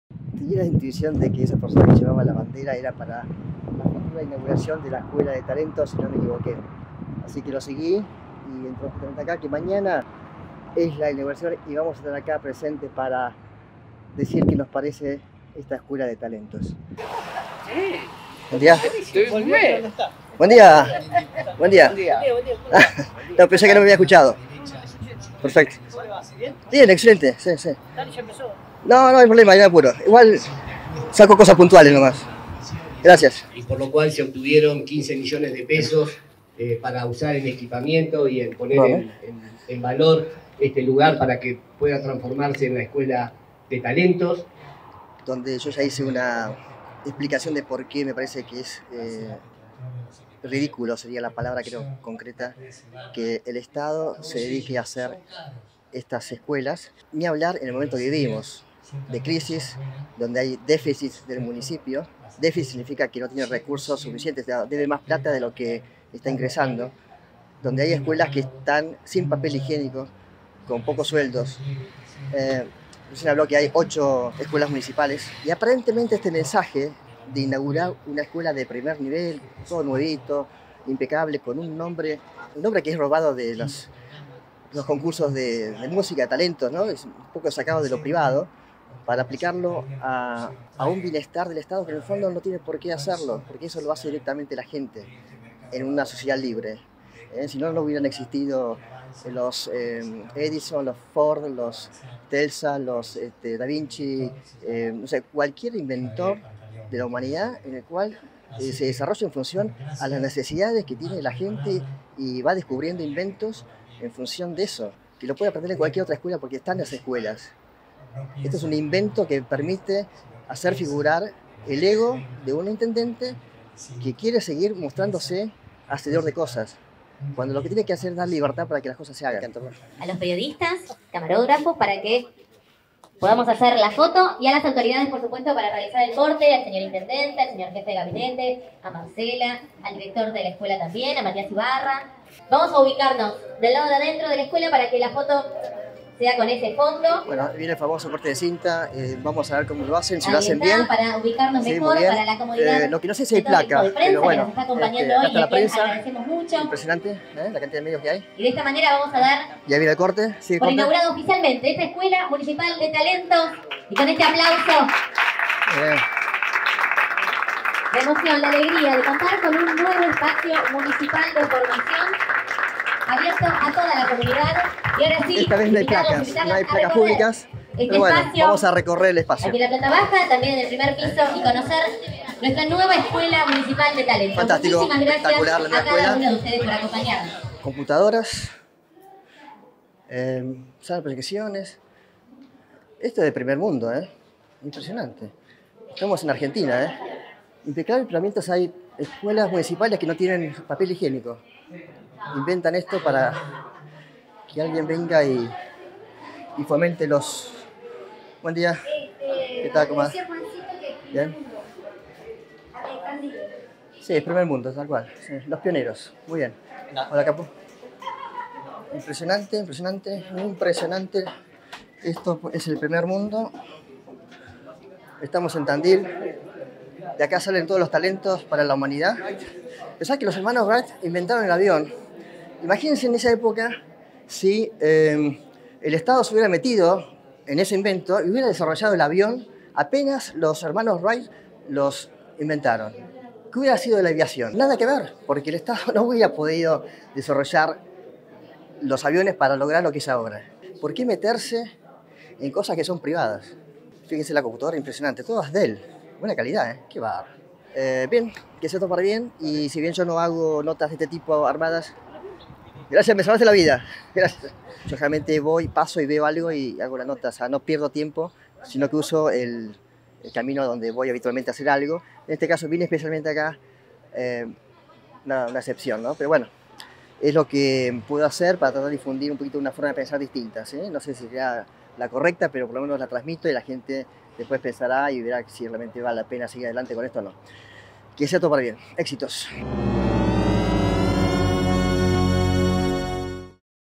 EL INDIVIDUO VS. EL ESTADO - ¿HACE FALTA UNA ESCUELA DE TALENTOS?
El intendente es muy vivo, porque sabe que este mensaje de inauguración de una escuela llamada de ¨Talentos¨ llega a la mayoría de la gente que no tienen grandes necesidades de educación. En cambio los que si tienen necesidades de mejorar la infraestructura o sueldos de escuelas municipales, no ve este anuncio de riqueza educativa, porque no es su realidad que está marcada por la pobreza dentro del mismo municipio. Sería comparable a unos padres que le entregan las mejores opciones de educación a un hijo, mientras que a los demás hijos los mantiene con lo mínimo. Sería muy humillante e injusto que esto se diera en una familia, lo mismo pasa en nuestro municipio.